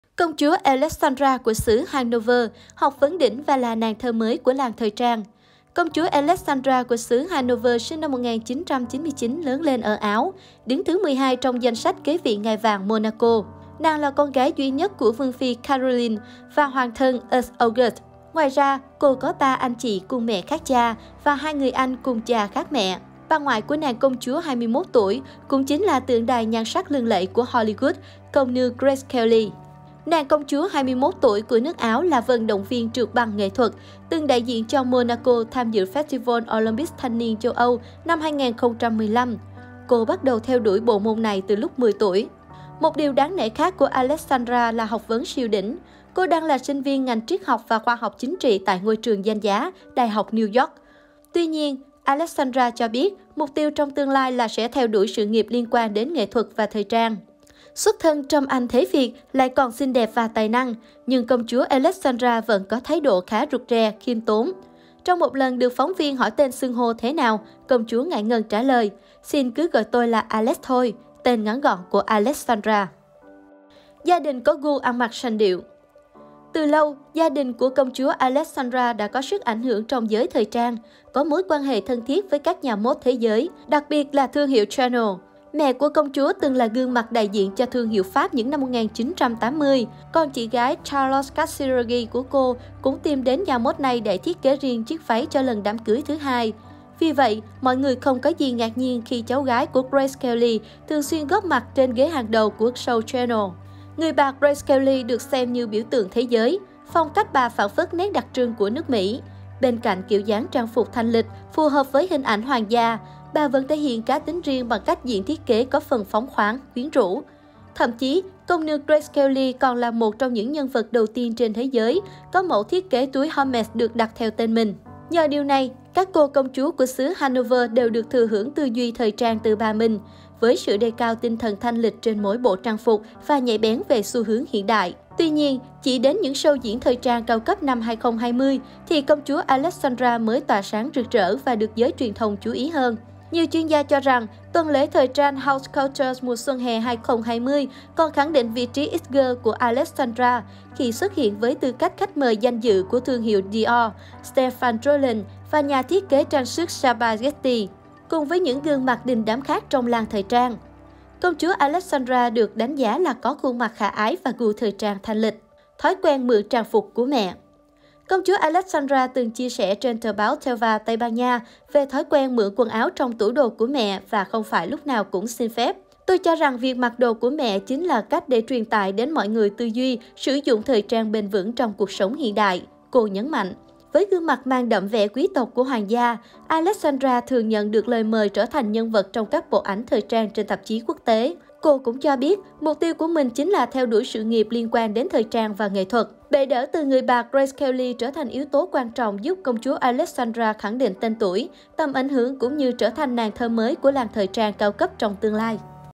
Công chúa Alexandra của xứ Hanover | Học vấn đỉnh và là nàng thơ mới của làng thời trang
Công chúa Alexandra của xứ Hanover sinh năm 1999 lớn lên ở Áo, đứng thứ 12 trong danh sách kế vị ngai vàng Monaco. Nàng là con gái duy nhất của Vương phi Caroline và Hoàng thân Ernst August. Ngoài ra, cô có 3 anh chị cùng mẹ khác cha và 2 người anh cùng cha khác mẹ. Bà ngoại của nàng công chúa 21 tuổi cũng chính là tượng đài nhan  sắc lừng lẫy của Hollywood - Công nương Grace Kelly.